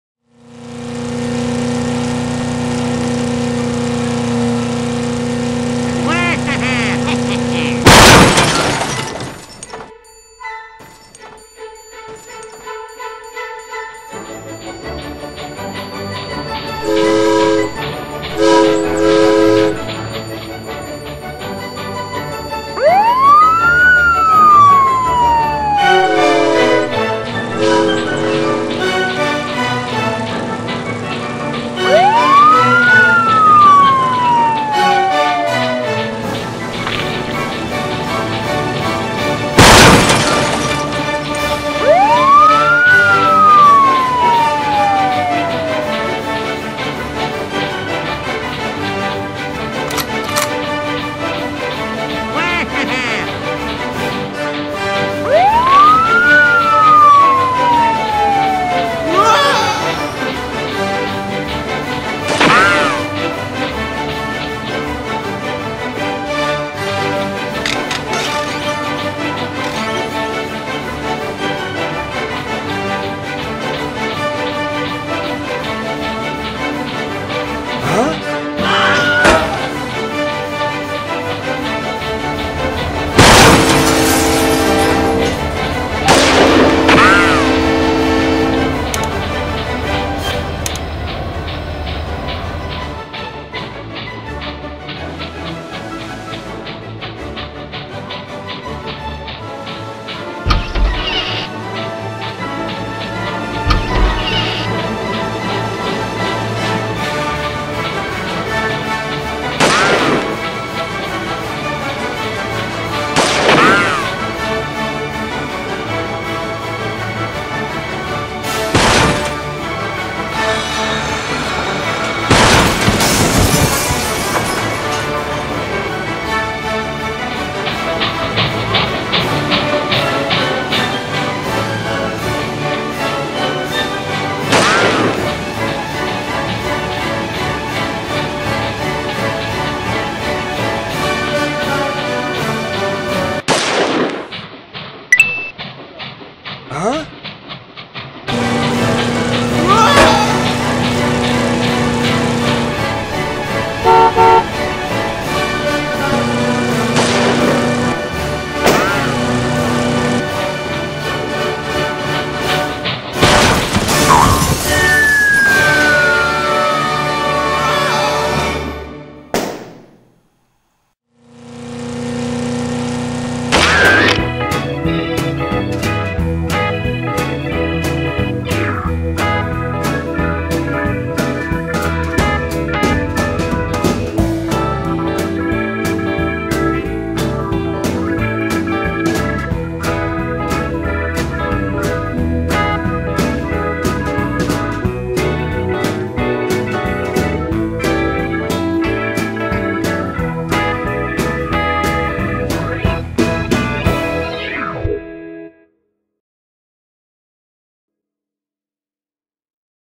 LEGO Train Chase - LEGO Police Chase Part 2
Bob the Robber has crashed his car! What will he do?